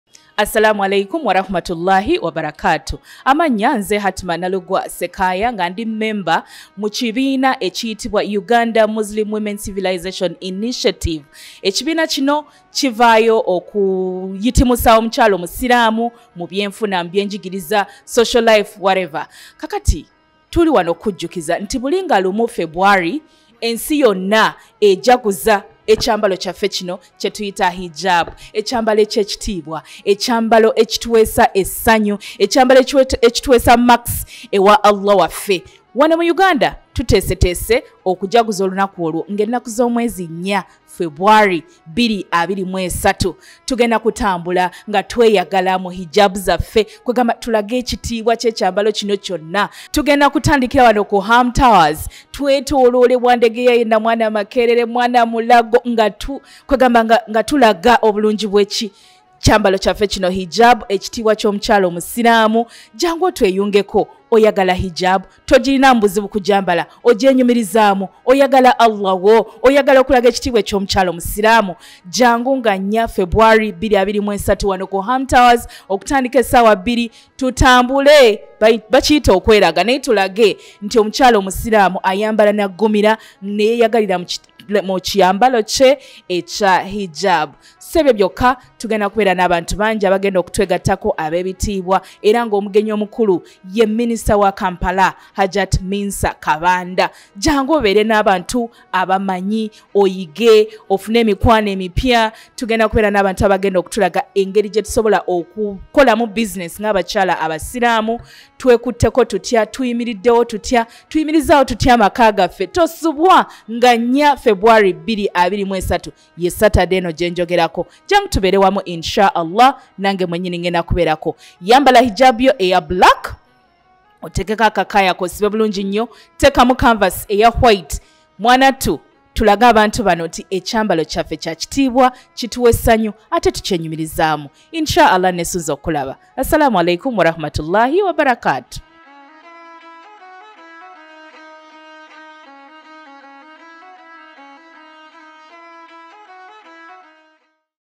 WORLD HIJAB DAY 2023 | HATMAH NALUGWA SSEKAAYA | UMWCI
A monthly show organized by Uganda Muslim Women Civilization Initiative (UMWCI) hosting outstanding women as guests for an open Muslim sisters get together and honest conversations about the things we desperately need to talk about, as women and as an ummah, its raw it's real, at times heavy, but always profoundly important.